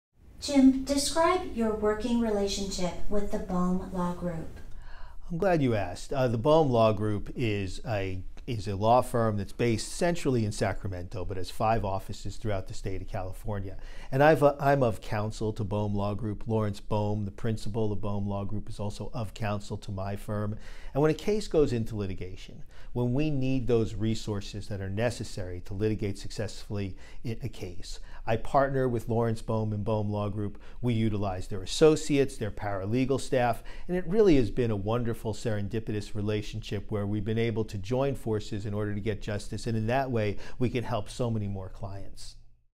James DeSimone and Bohm Law Group
Attorney James DeSimone describes his working relationship with Bohm Law Group and how they leverage each other's resources and experience to help their clients.